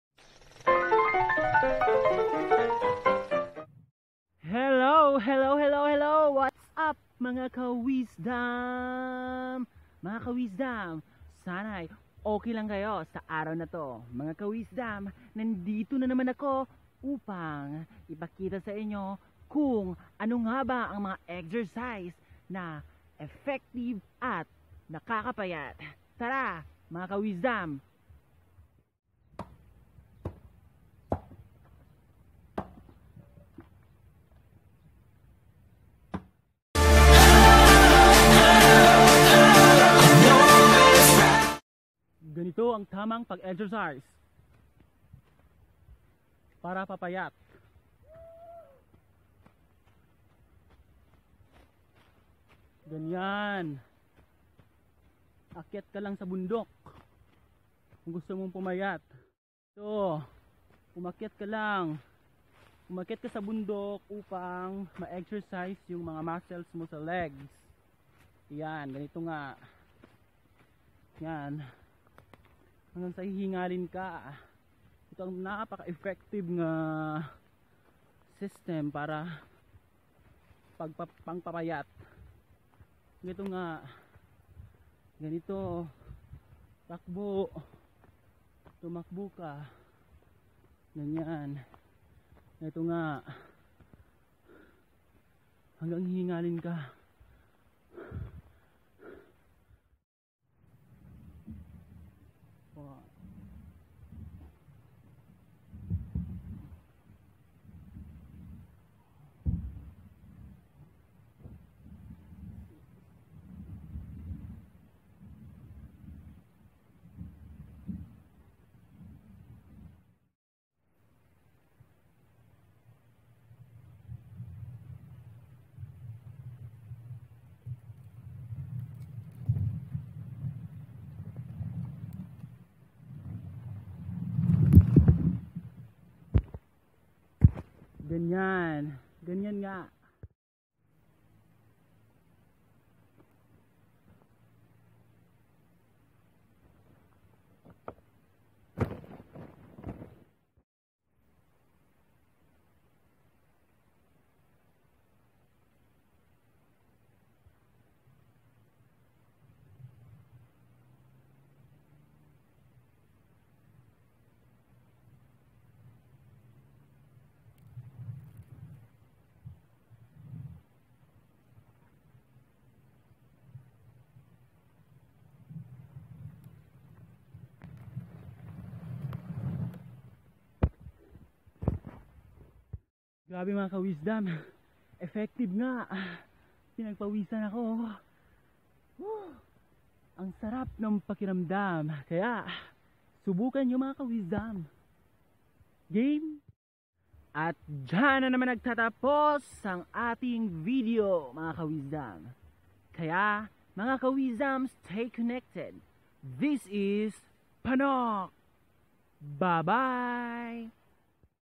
GANITO KAMI MAG EXERCISE SA BUKID (TUTORIAL)
hellow mga ka wisdom. .!! kmzta na kayo naway nasa mabuting mga kalagayan kayo.andito nanaman AKO mga ka wisdom upang mag bigay ng saya at kaunting aral sa inyong mga sarili.tara samahan nyo akung mag exercise Sa bukid mga ka wisdom...